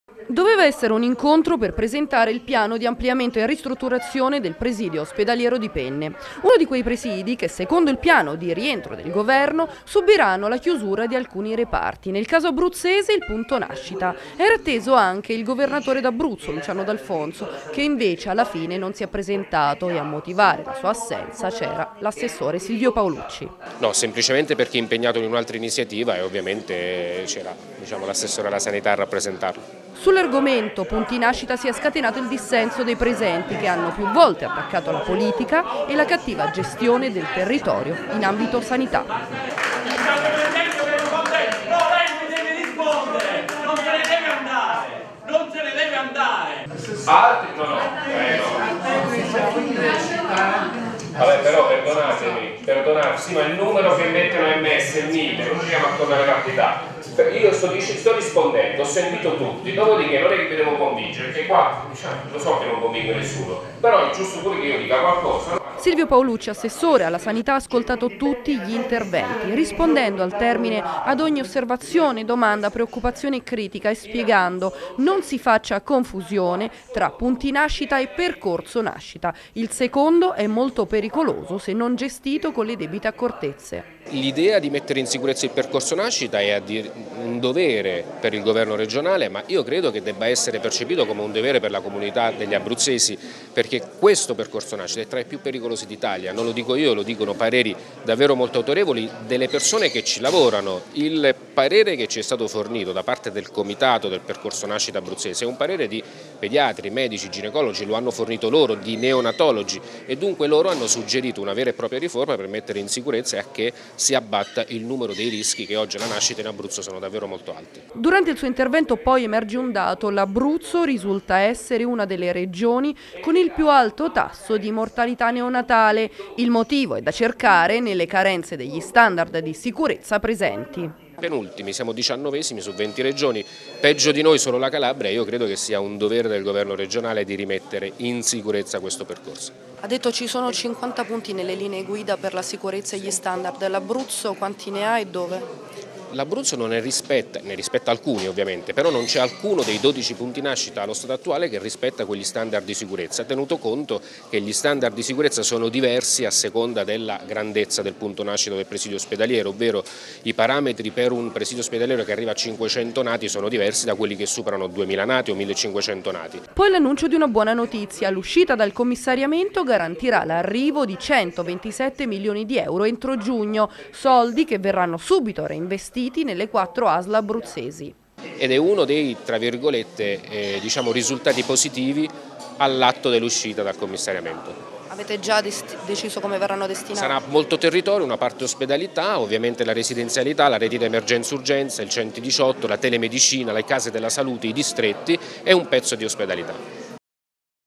Penne, la protesta sulla sanità
A Penne è stato presentato il piano di ampliamento e ristrutturazione  dell'ospedale San mnassimo, le istituzioni regionali,nel conrso della conferenza,sono state interrotte dalle proteste dei cittadini che continuano a chiedere che non venga chiuso il loro punto nascita.